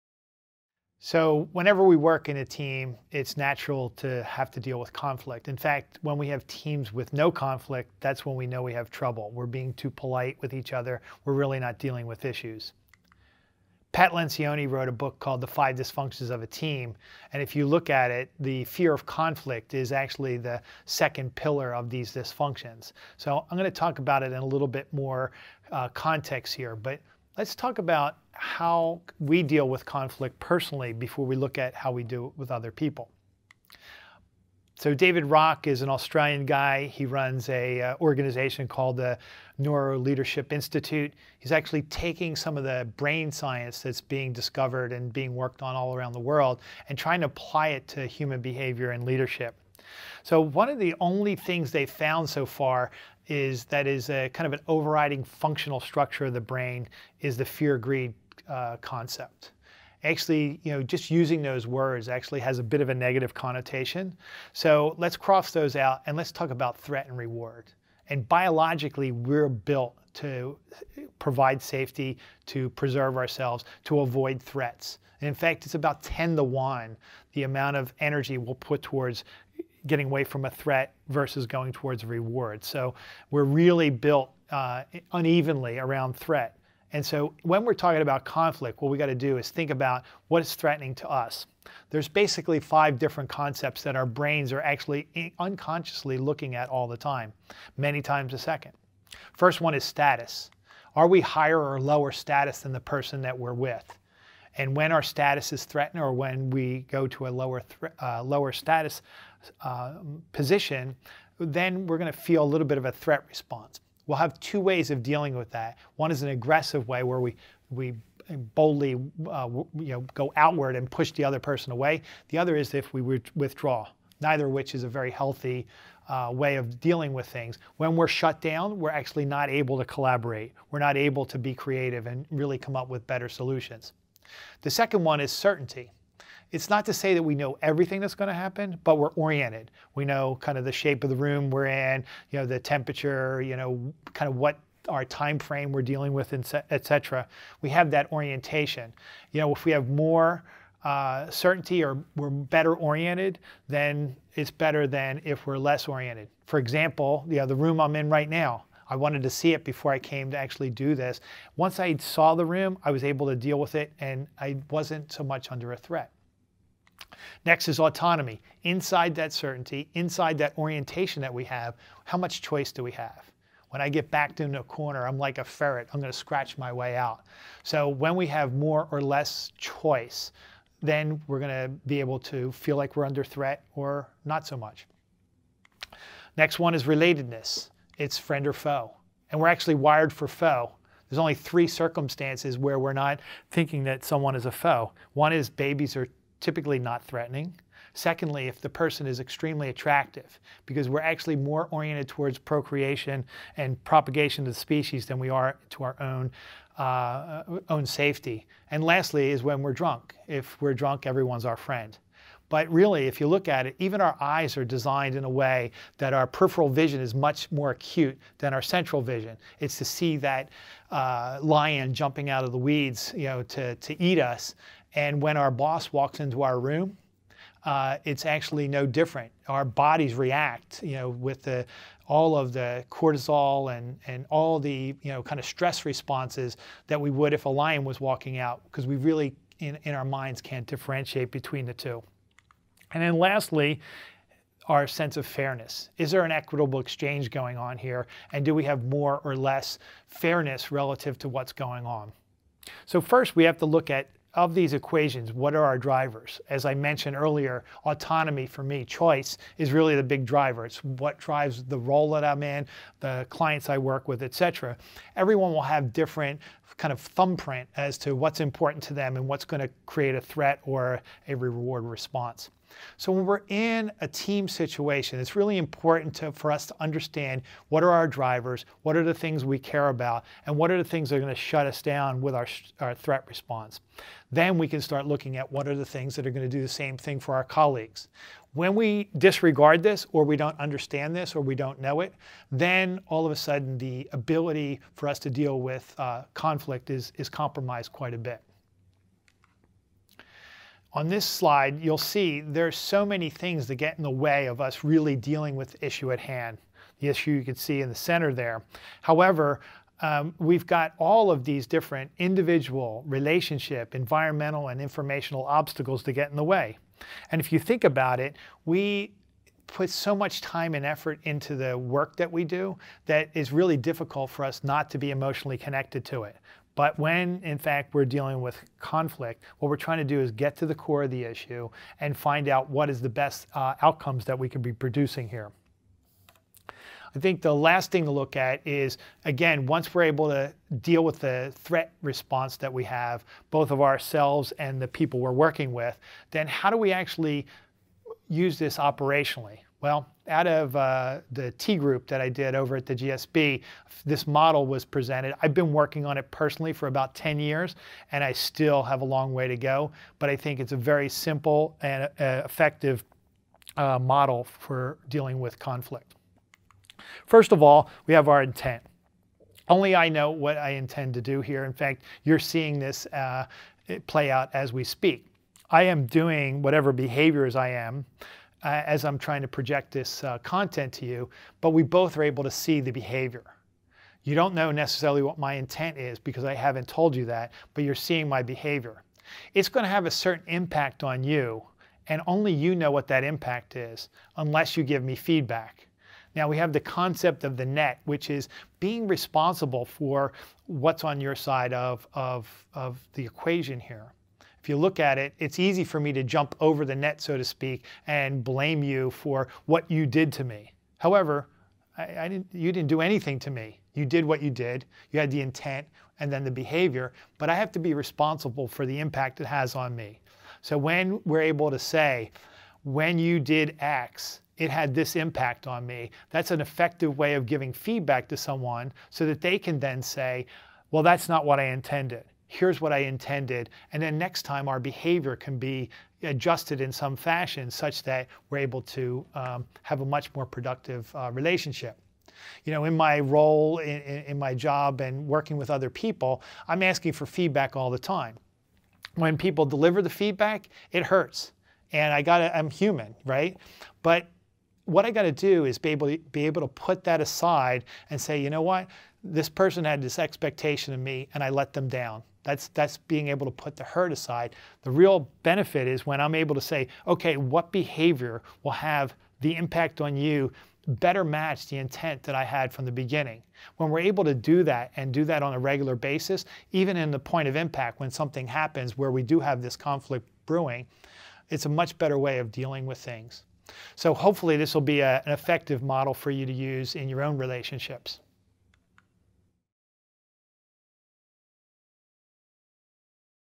Bill Tobin - Dealing With Conflict
Bill Tobin discusses what causes conflict and how to deal with it in a team